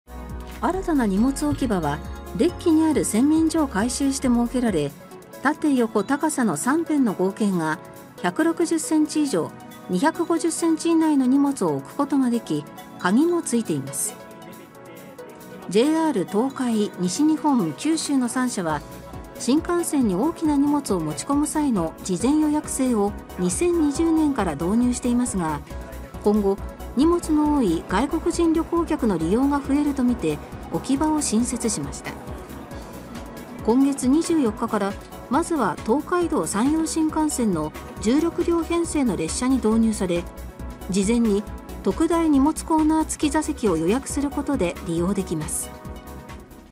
新幹線に大型荷物置き場新設置 旅行客増見込み(2023年5月9日)
外国人旅行客などの増加を見越し、新幹線の車内に大型の荷物置き場が新たに設置されました。

　新たな荷物置き場はデッキにある洗面所を改修して設けられ、縦・横・高さの3辺の合計が160センチ以上250センチ以内の荷物を置くことができ、鍵も付いています。

　JR東海、西日本、九州の3社は新幹線に大きな荷物を持ち込む際の事前予約制を2020年から導入していますが、今後、荷物の多い外国人旅行客の利用が増えるとみて、置き場を新設しました。

　今月24日から、まずは東海道・山陽新幹線の16両編成の列車に導入され、事前に「特大荷物コーナーつき座席」を予約することで利用できます。